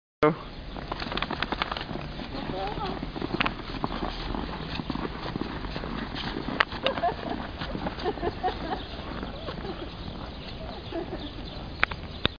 Blizzard Walk 2010
Sarah and Chester go for a walk during a snowstorm in Kansas City